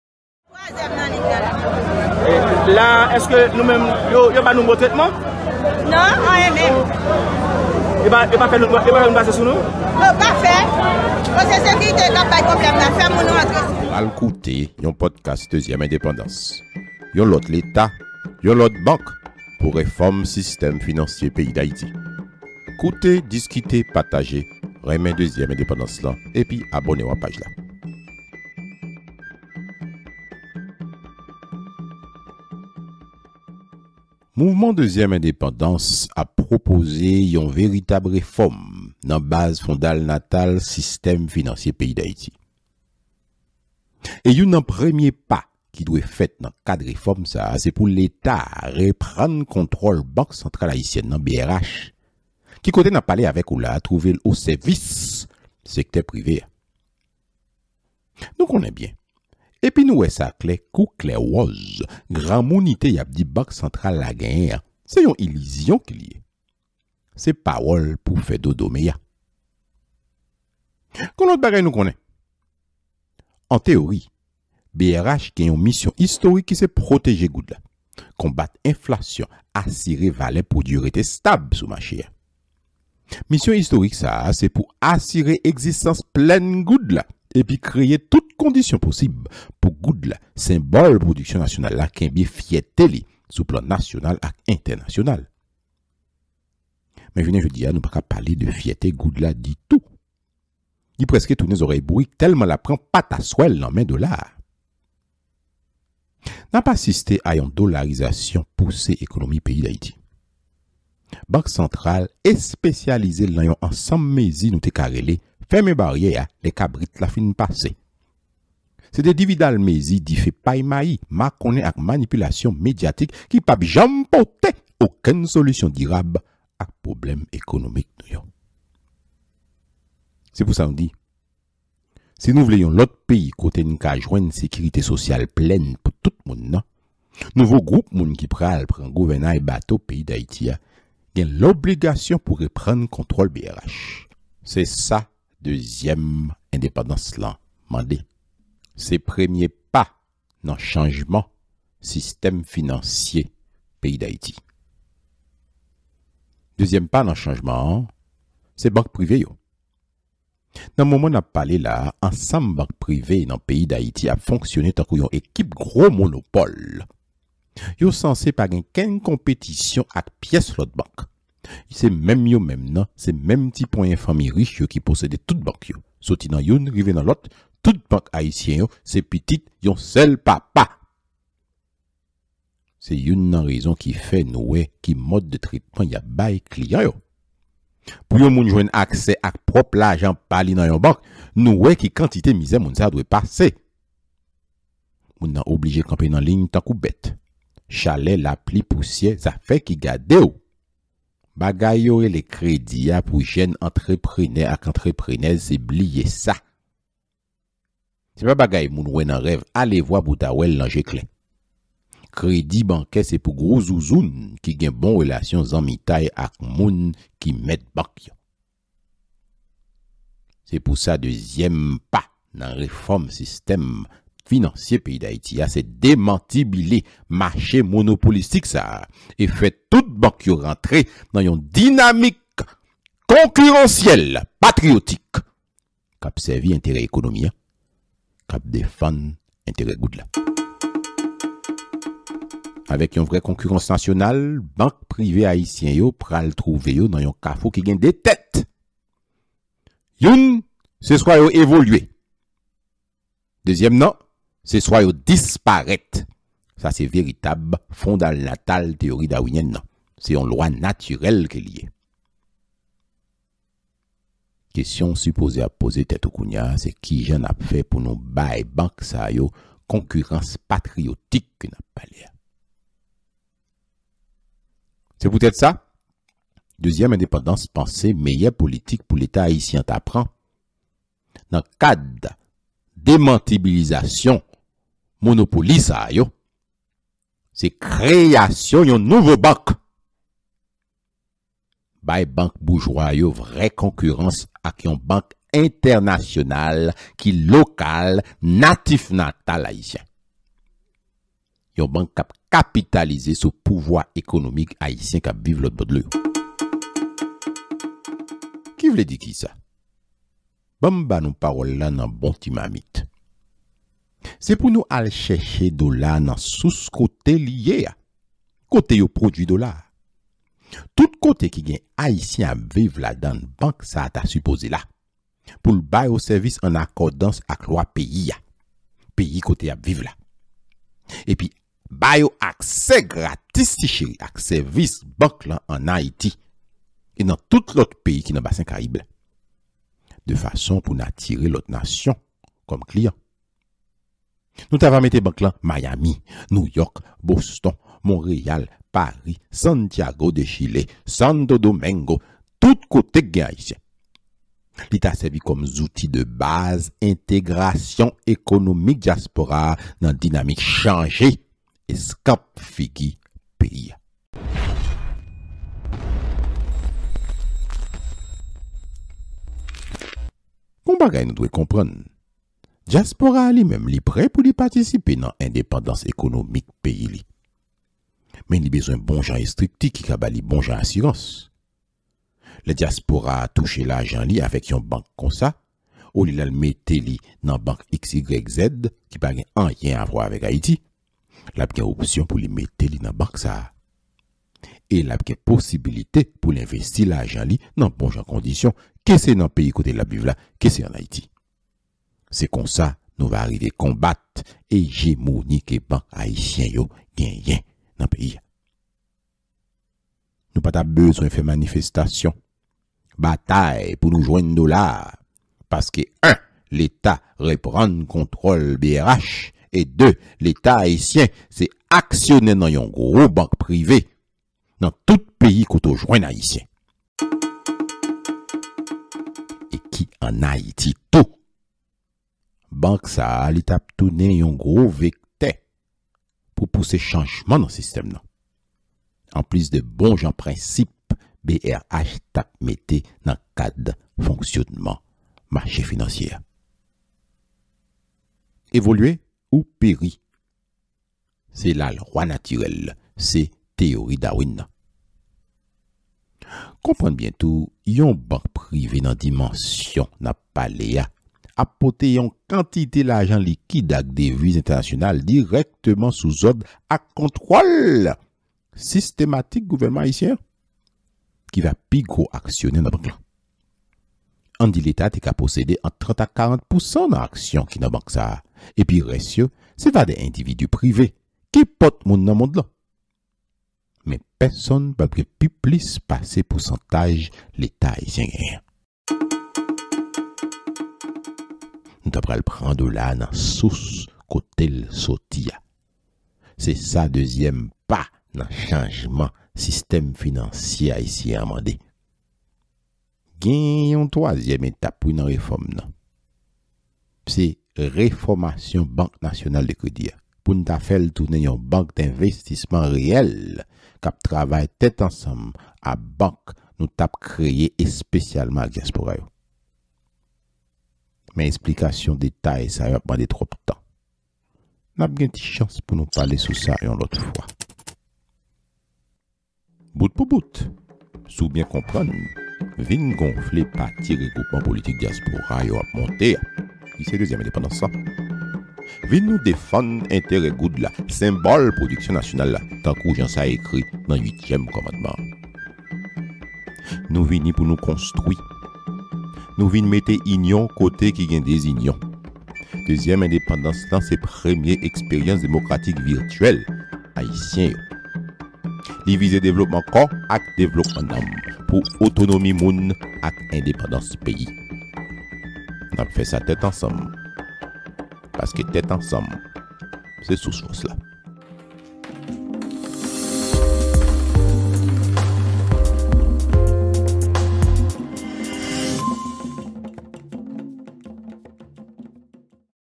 Demantibile yo!
Moun pase twòp mizè an Ayiti nan zafè al cheche kòb la bank. Sak pi rèd la. Se pou pwòp kòb pa w. N ap denonse, men tou n ap pwopoze bon jan solisyon.
Koute. Pataje. Abòne w a chanèl la.